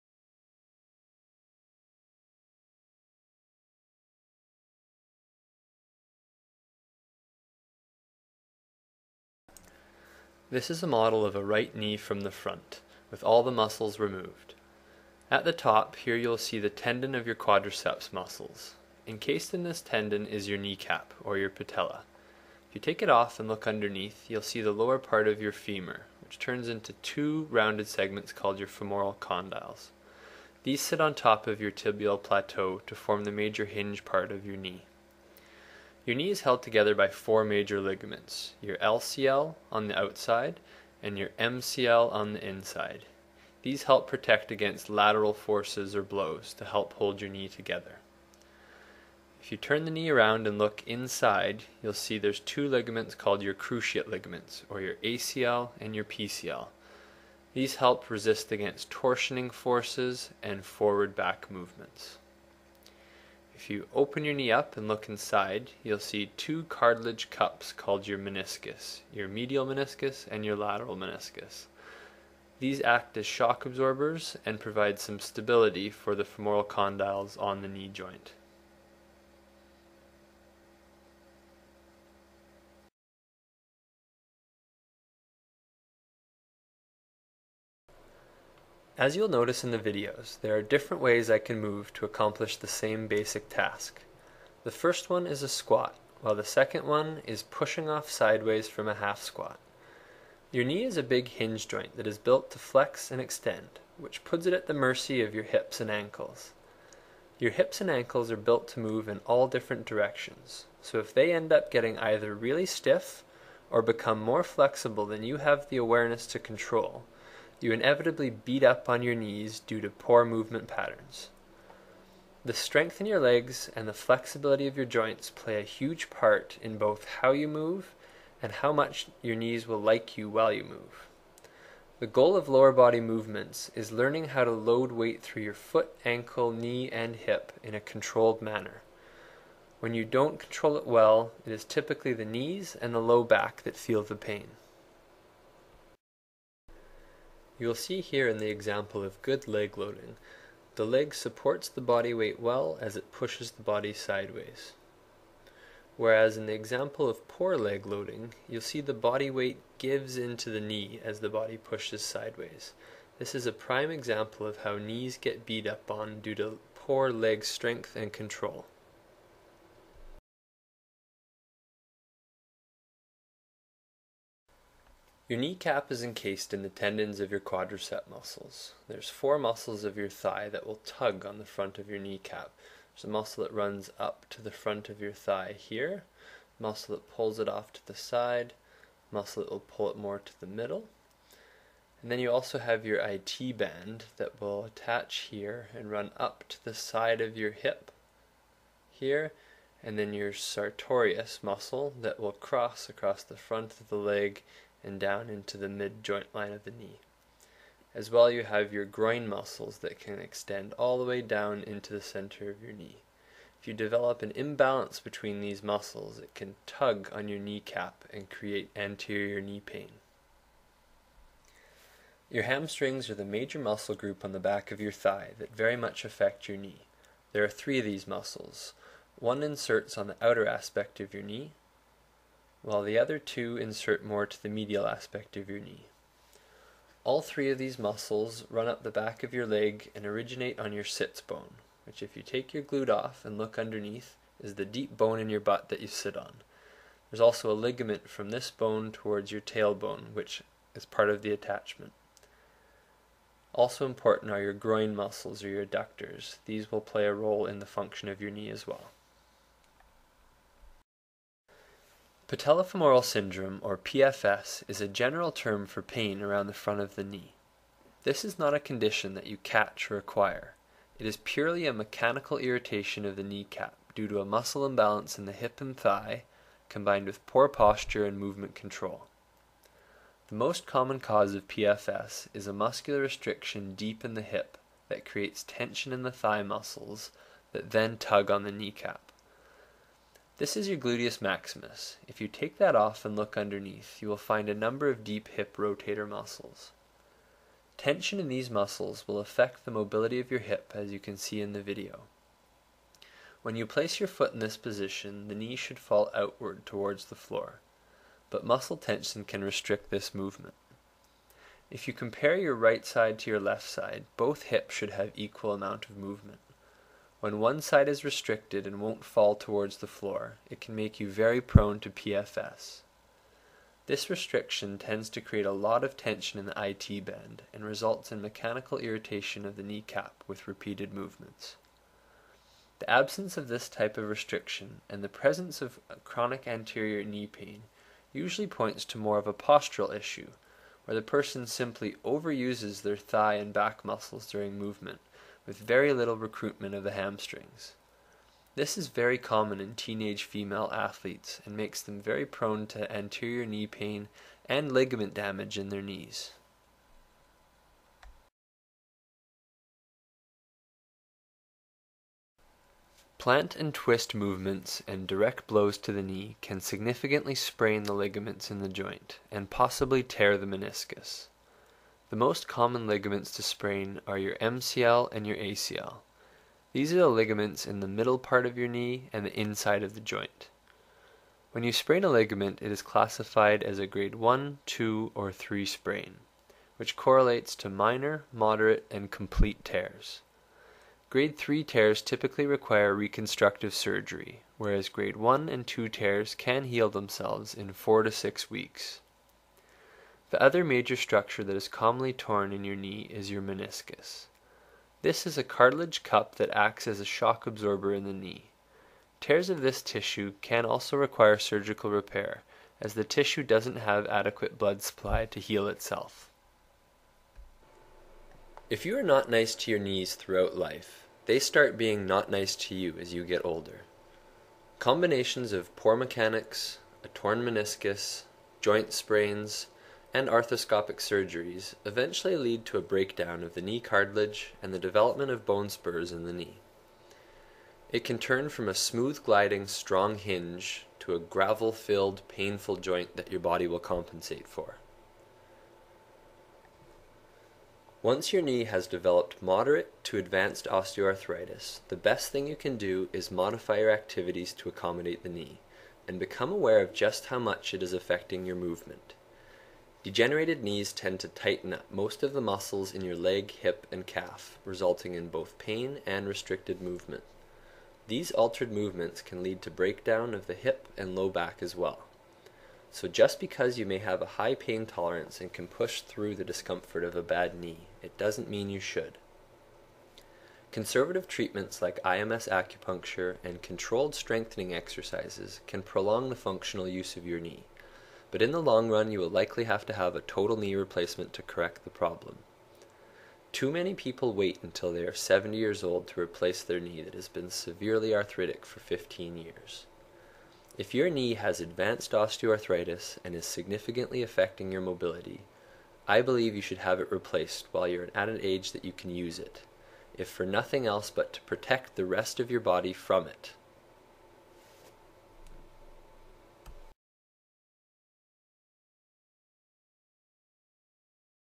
Why Knees Hurt
Physiotherapist Brent Stevenson from WhyThingsHurt.com explains to root cause of a lot of knee pains